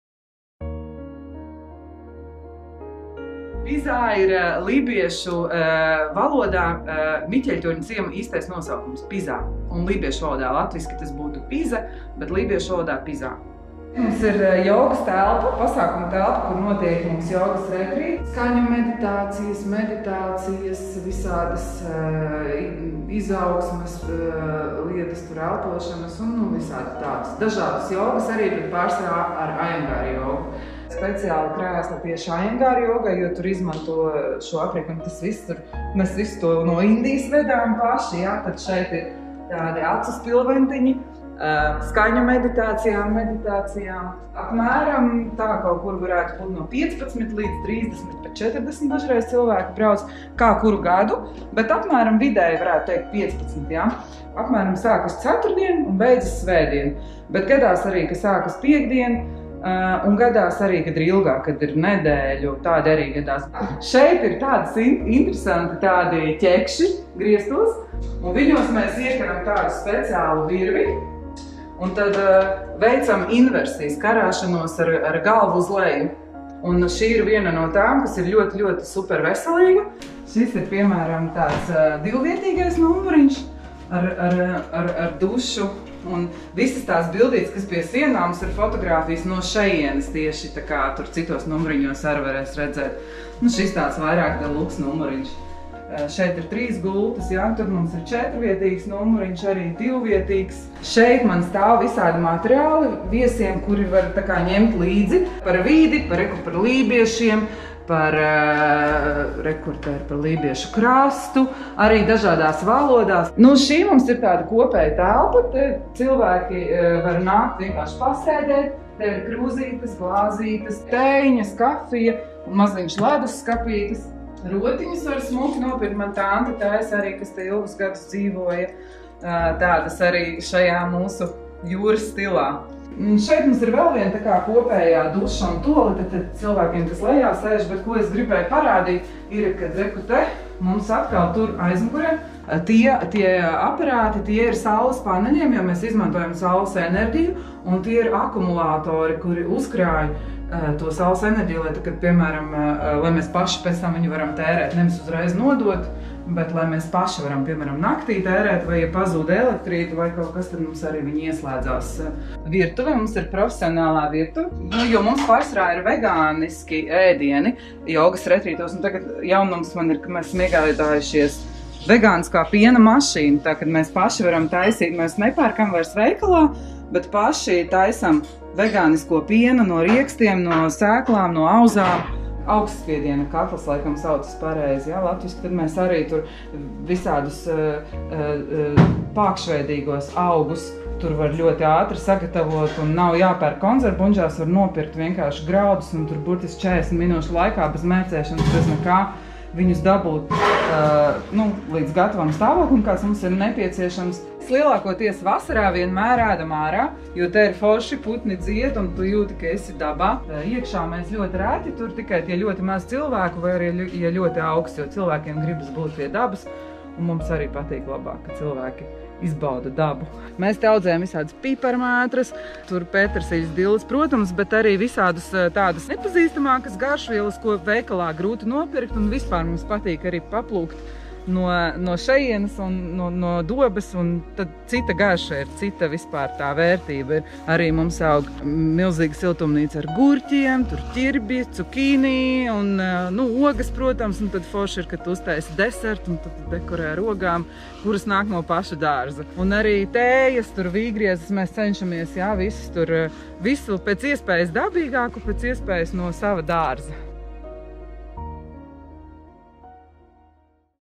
Zaļā saimniekošana brīvdienu mājā "Pizā"
Brīvdienu mājas “Pizā” saimniece Ulla Fraser saimnieko Lībiešu krastā, pārsimts metrus no Baltijas jūras. Vārds “Pizā” ir lībiskais Miķeļtorņa nosaukums. Ulla piedāvā jogas retrītus, meditācijas, izaugsmes un inversiju trenniņus nelielām interesentu grupām nedēļas nogalēs. Šim mērķim ir iegādāts speciāls aprīkojums. Viesu ērtībām ir atsevišķas telpas, kur var atpūsties, iemalkot tēju, palasīt grāmatas un iegādāties vietējo amatnieku ražotos suvenīrus. “Pizā” ir profesionālā virtuve, kur gatavo vēgāniskus ēdienus un dzērienus, bet augstpiediena katlā sagatavo pākšveidīgo graudus. Maltīti viesi ietur ārā – dabā. Dārzā aug garšvielas un dārzeņi, kurus izmanto ēdiena gatavošanā.
Zaļā sertifikāta vērtēšana un attīstība notiek ar Latvijas vides aizsardzības fonda atbalstu.

Zaļā sertifikāta vērtēšana un attīstība notiek ar Latvijas vides aizsardzības fonda atbalstu. “Zaļā sertifikāta” aktualizēšana, informācija un pretendentu pārbaude 2024. gadā” / Nr.: Nr. 1-08/59/2024